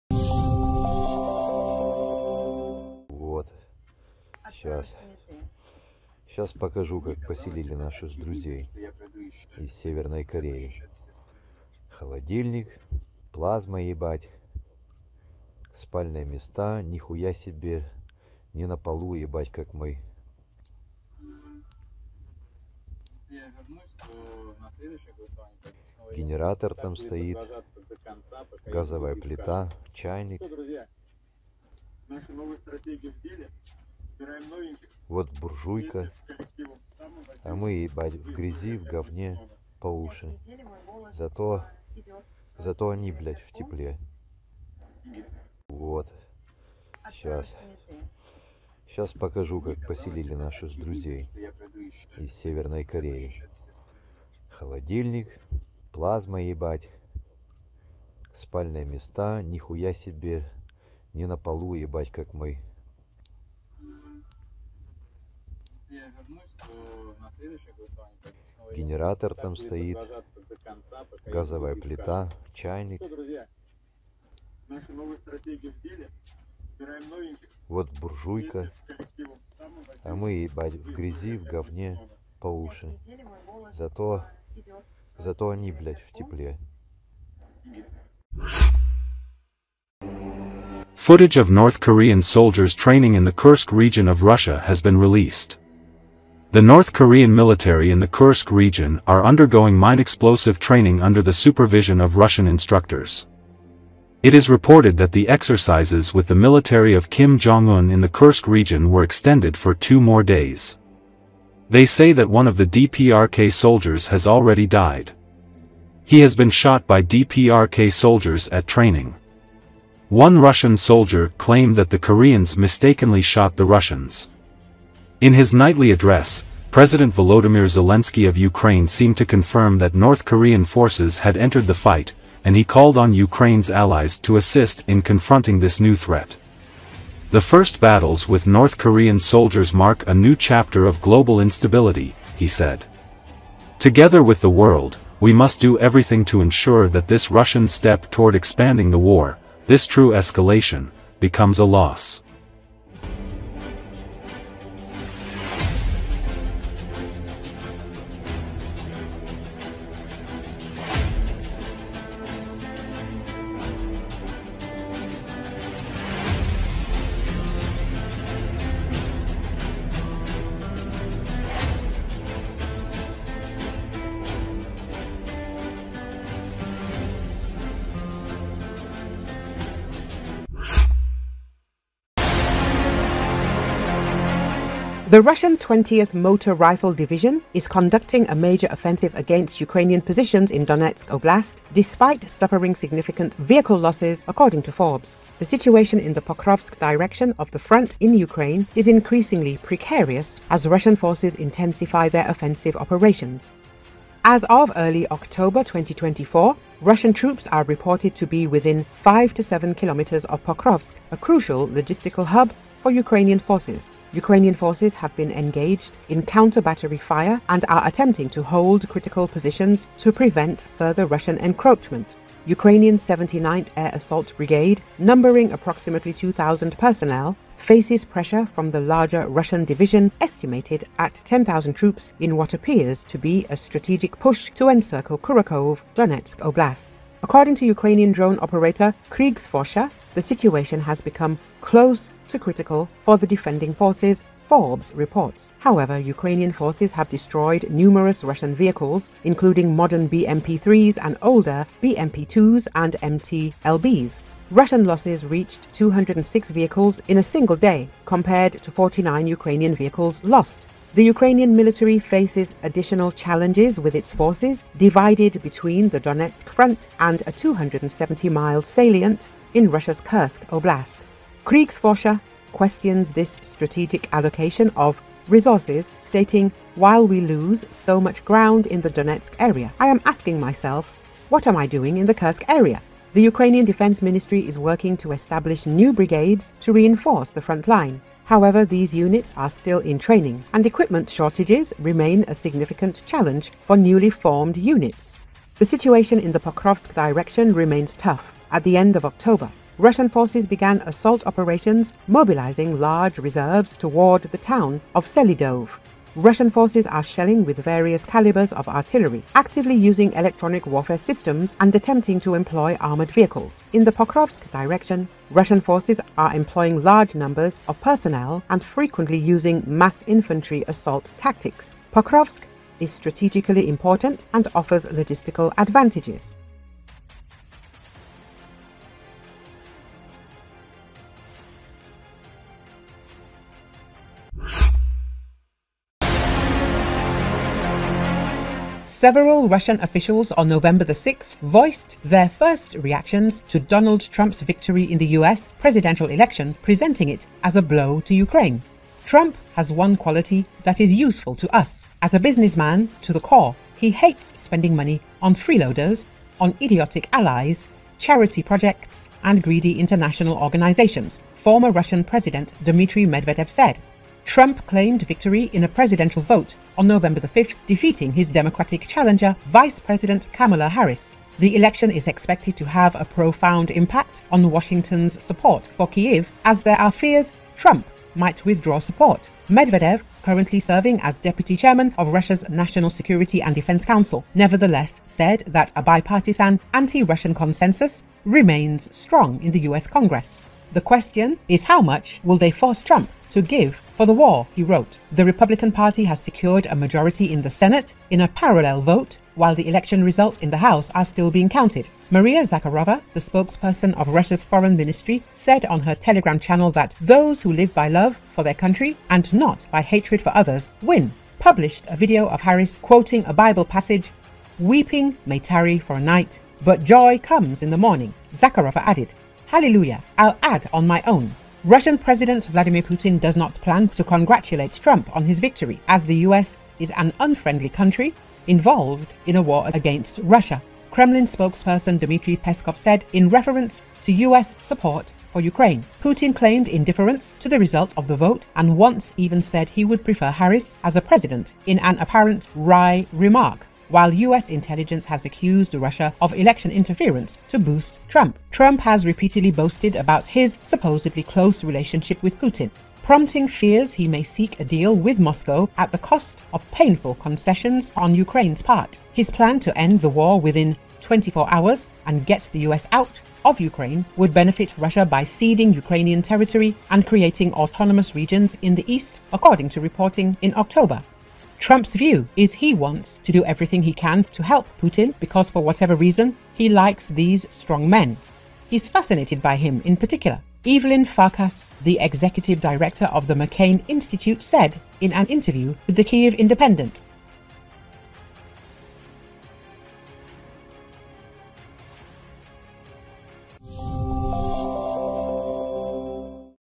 Russians show North Korean soldiers' camp and protest - "We are sleeping in mud"
A Russian army soldier has shared a footage of the place where North Korean soldiers were stationed. The Russian soldier said that the Korean soldiers were kept warm and in better conditions than the Russians, adding that Russians stayed in a cold and muddy place. The images show a stove and household appliances for heating in the place where the Korean soldiers are being held.